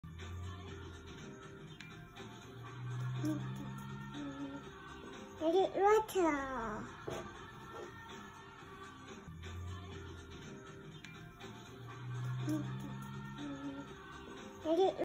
Haley is using Crayola Washable project Paint Classic!
Haley loves painting but hard to stop her now.
oh no!